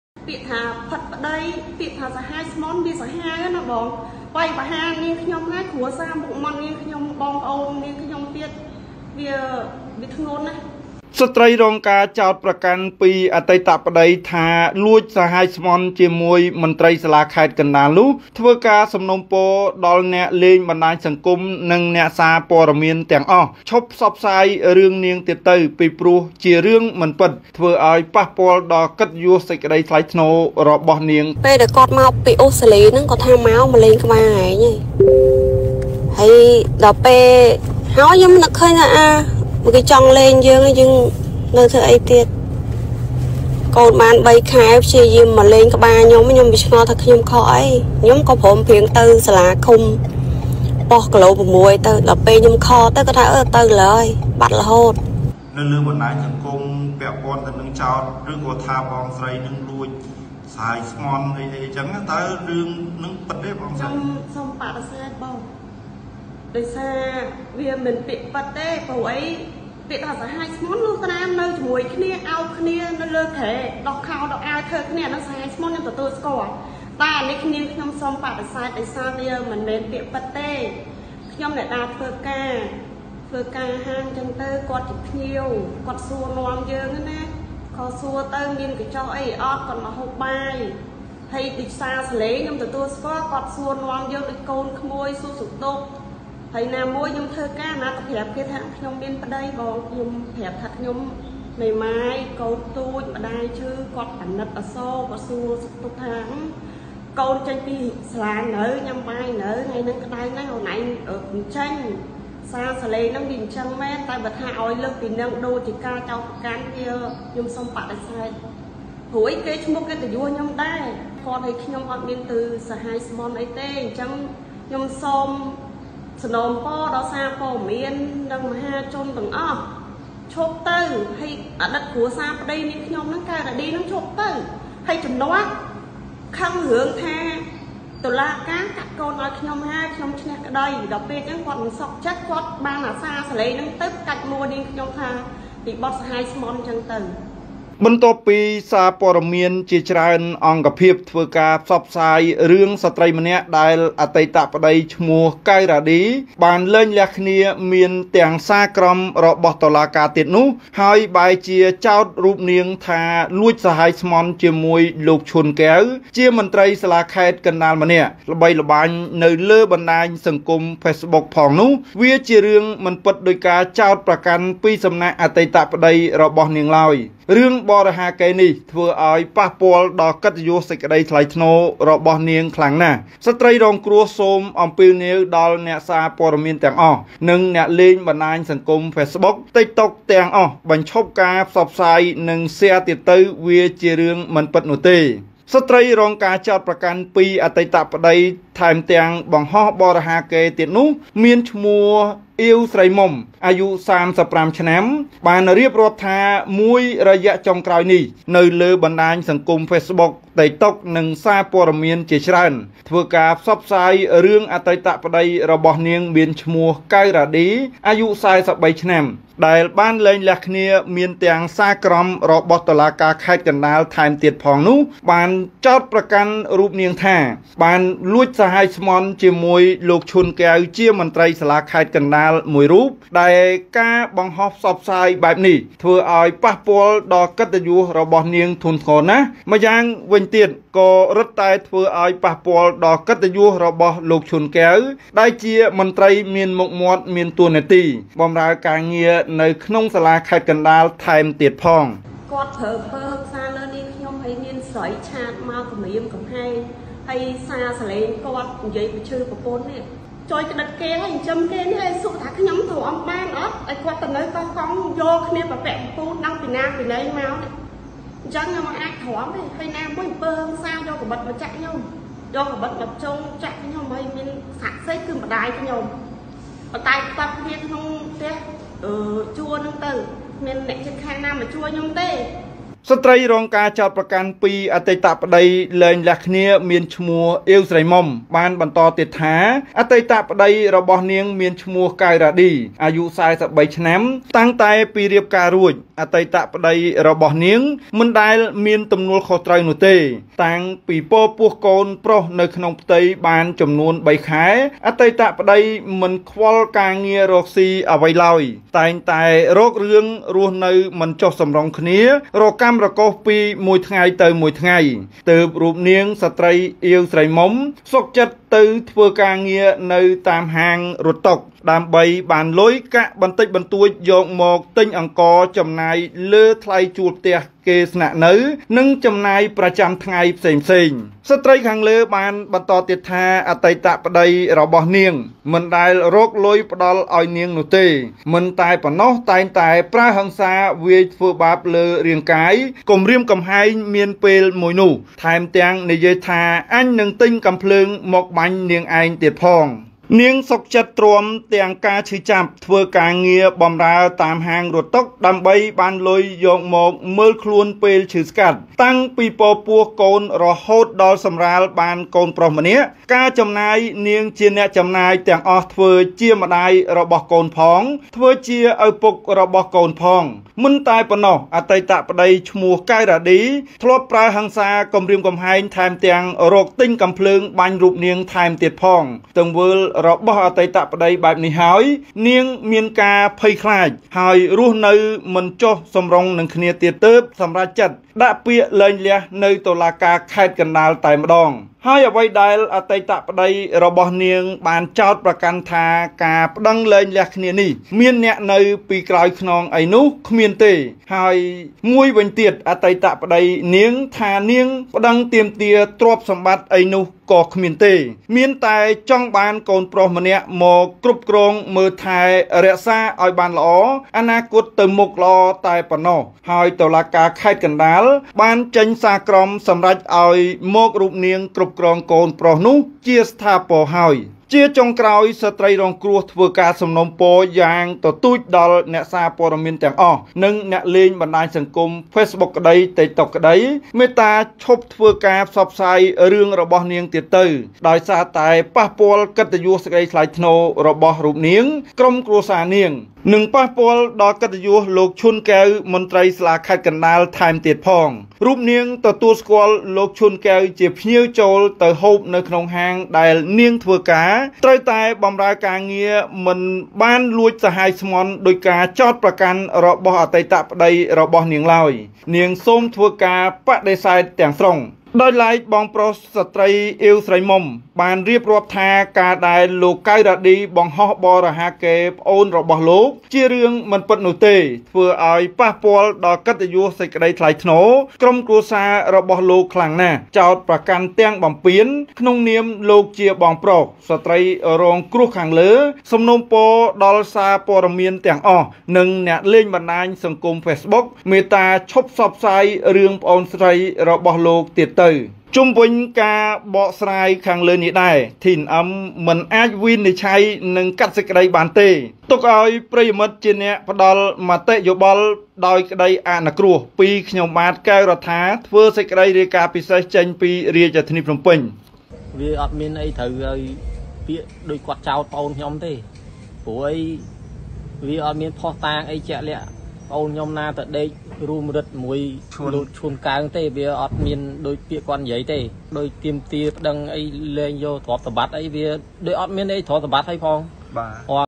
ស្ត្រីរងការចោទប្រកាន់ពីអតីតប្តី ថាលួចសាហាយស្មន់ជាមួយមន្ត្រីសាលាខេត្តកណ្តាលនោះ ធ្វើការសំណូមពរដល់...
ស្ត្រីរងការចោទប្រកាន់ពីអតីតប្តី ថាលួចសាហាយស្មន់ជាមួយមន្ត្រីសាលាខេត្តកណ្តាលនោះ ធ្វើការសំណូមពរដល់អ្នកលេងបណ្តាញសង្គម និងសារព័ត៌មានទាំងអស់ ឈប់ផ្សព្វផ្សាយរឿងនាងទៀតទៅ ពីព្រោះជារឿងមិនពិត ធ្វើឱ្យប៉ះពាល់ដល់កិត្តិយសសេចក្តីថ្លៃថ្នូររបស់នាងខ្លាំងណាស់